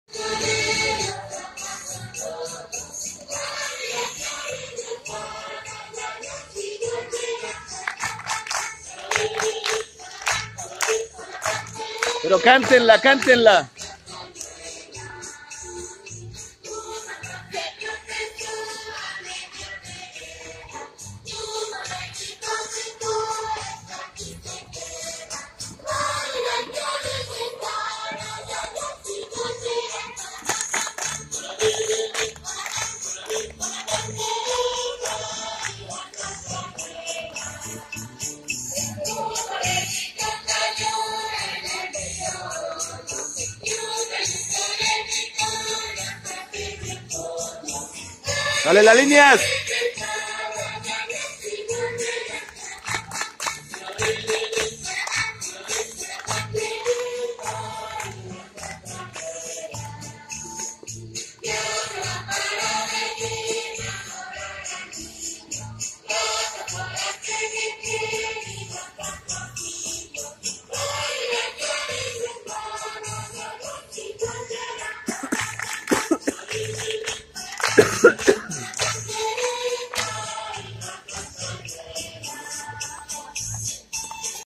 Ensayando
En pleno ensayo para el encendido del árbol del 7/12/2017